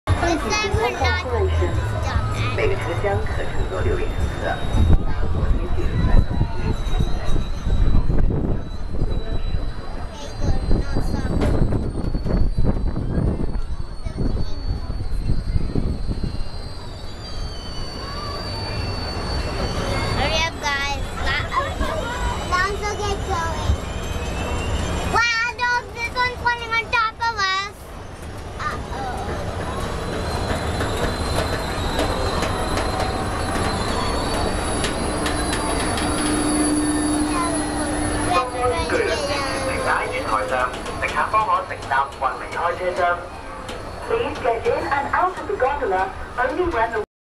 AW, Rahil: Rahil takes a lap on the Ocean Park Hong Kong ferris wheel
Ocean Park
Hong Kong
26 March 2011